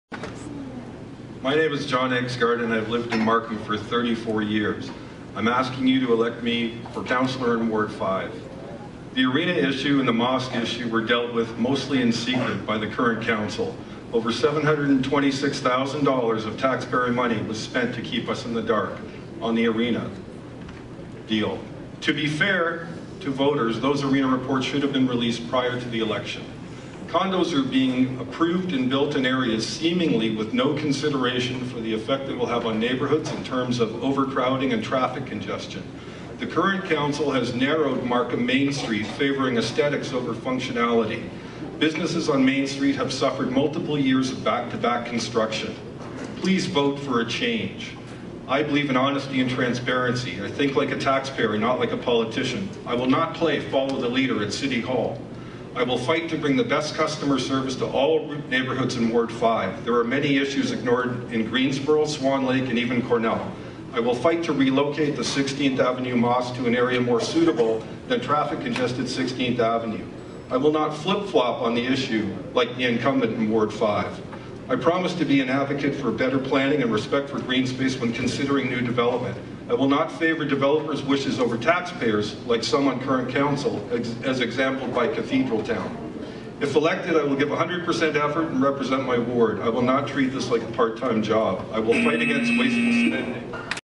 John Egsgard Markham Ward Councillor Candidate at All Candidates Meeting at Seneca College
John Egsgard Markham Regional Councillors Candidates 90-sec speeches at Meet'n'Greet  All Candidates Meeting at Seneca College